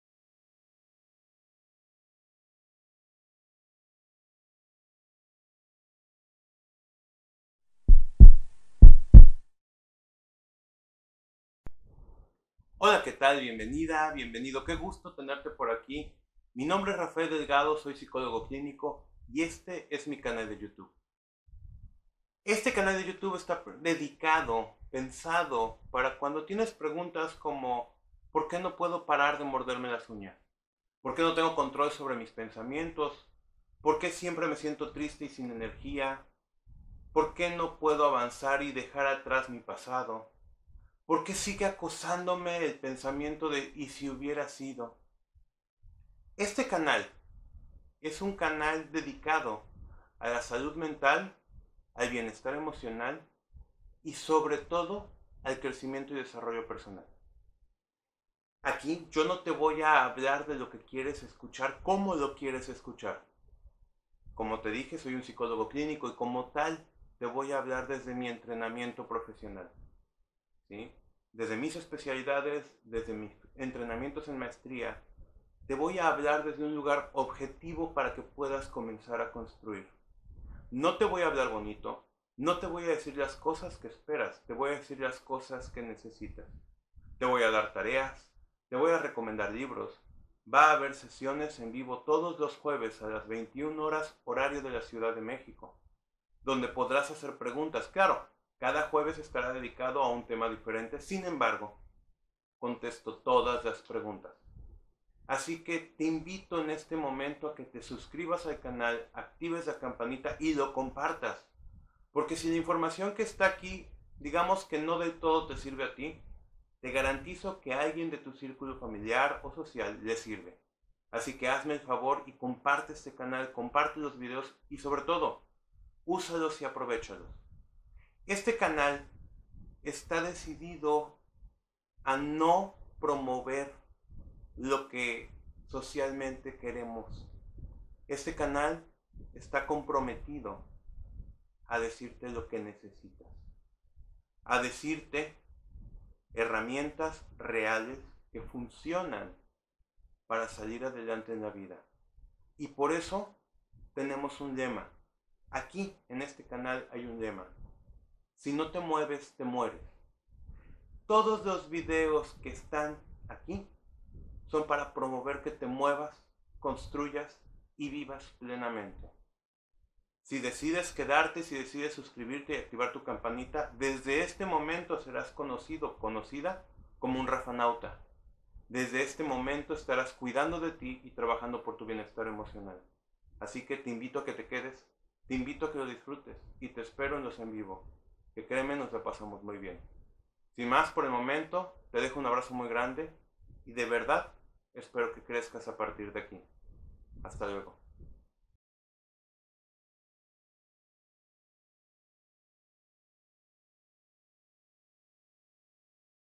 BIENVENIDO A MI CANAL Trailer: ¿Qué encontraré en este canal?
Te doy la mas cálida bienvenida a este, mi canal.
Soy Rafael Delgado, psicólogo clínico y este es un espacio dedicado a promover la salud mental y emocional desde una postura objetiva.

Deja tu comentario, recuerda que este espacio es para apoyarnos, aclarar dudas, opinar y debatir.

¡Gracias por ser parte de esta comunidad!